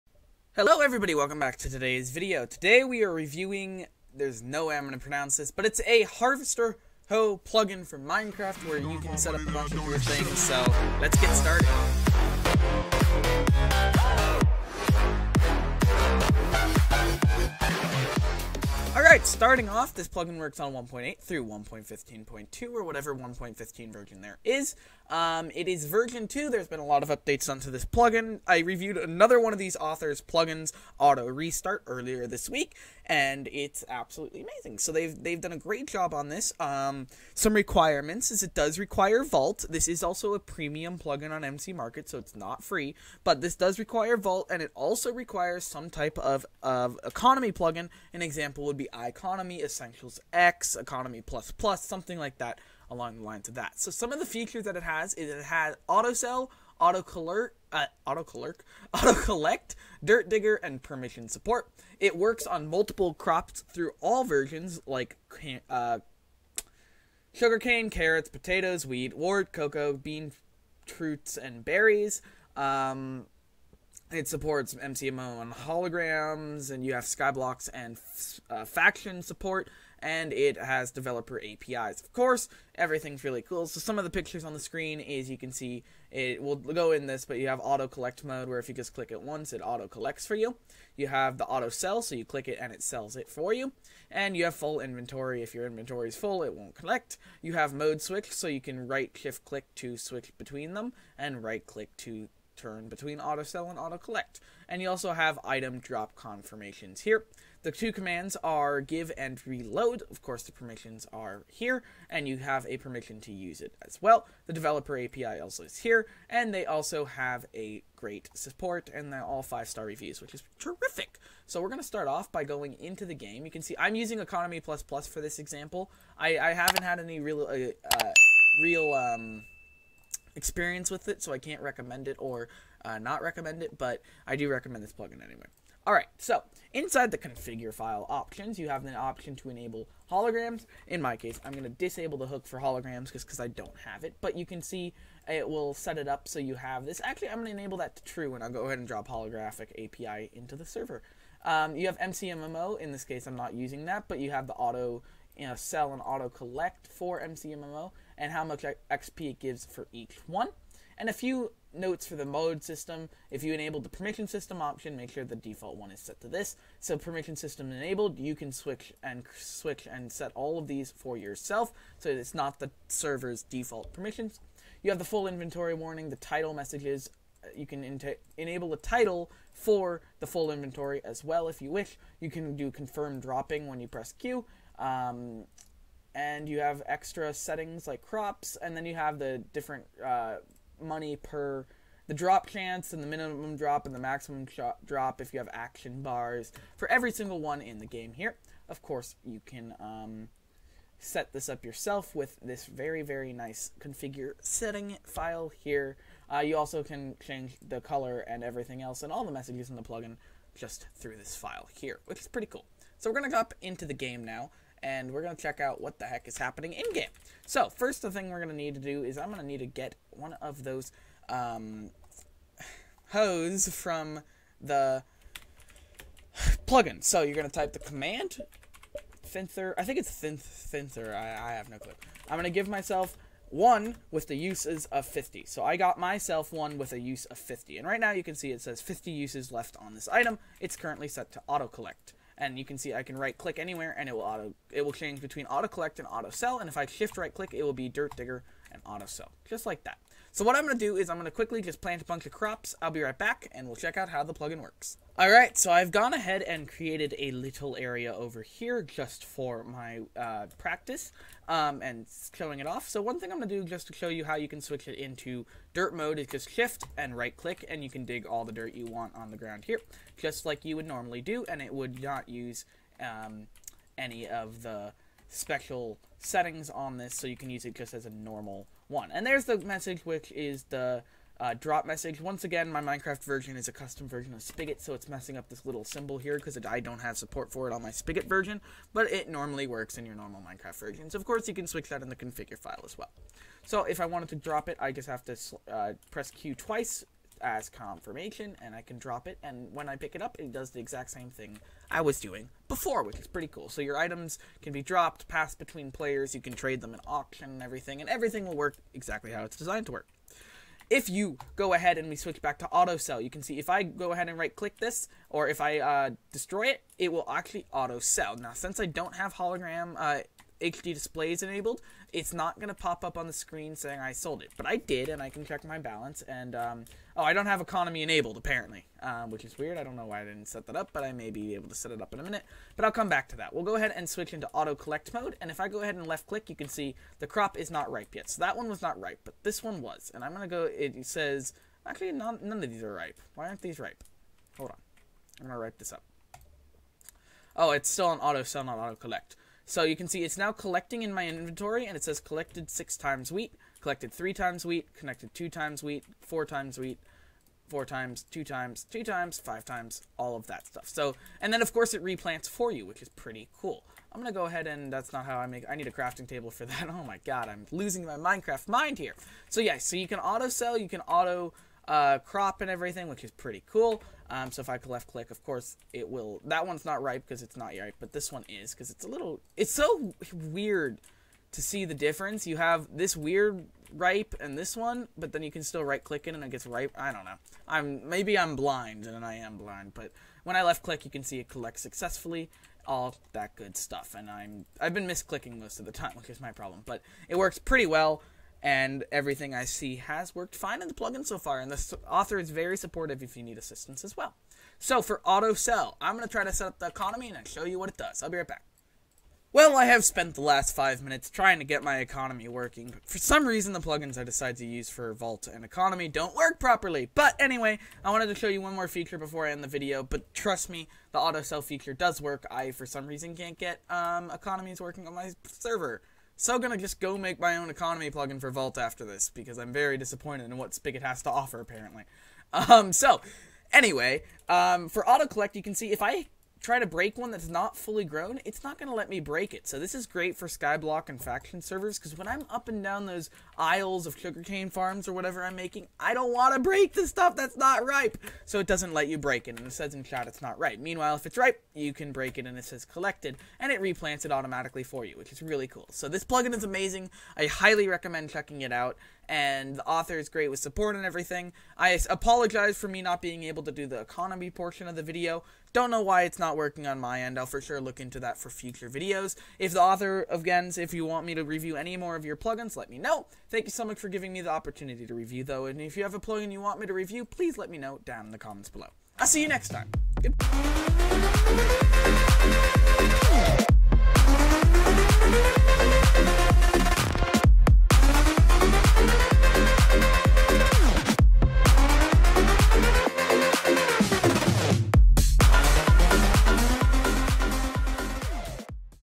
Scyther - Plugin Tutorial/Overview
Welcome to today's PluginTutorial featuring the Scyther plugin from MC-Market. As this is not a BGHDDevelopment plugin we are unable to provide support for it in the comments, please contact the author on the MC-Market page if you require support.